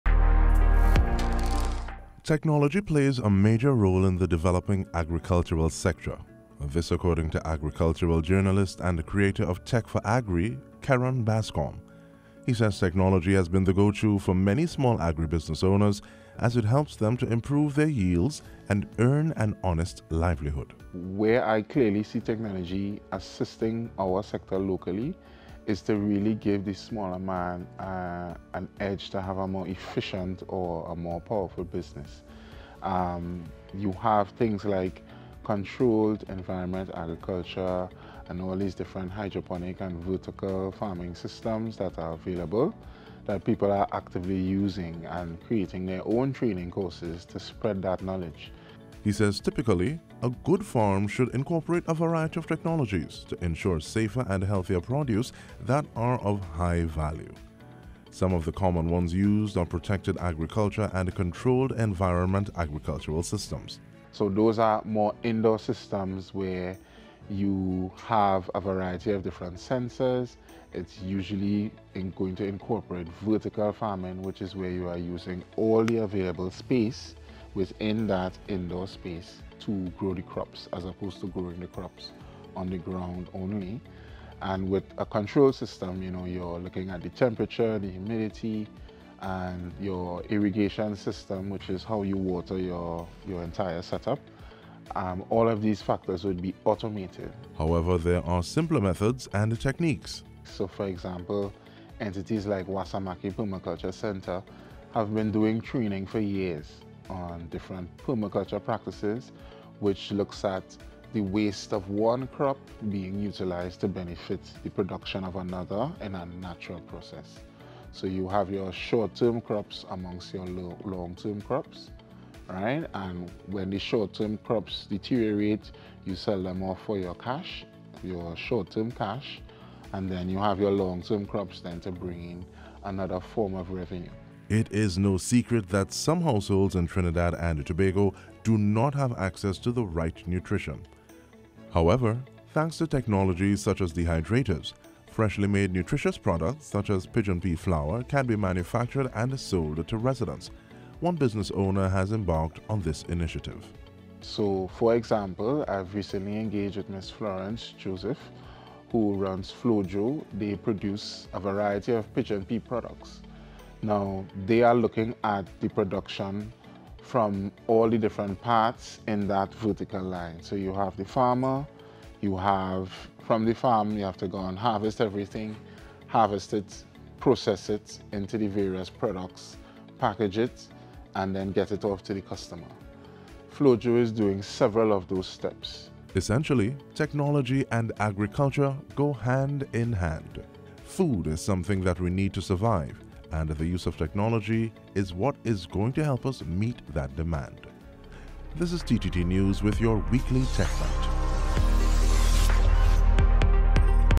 Tech Bites - Technology In Agriculture
Oftentimes, when we think of technology, our electronic devices come to mind. However, technology is more than just phones and computers.

Have you ever considered the role it plays in the agricultural sector?

Find out more in today's Tech Bites feature.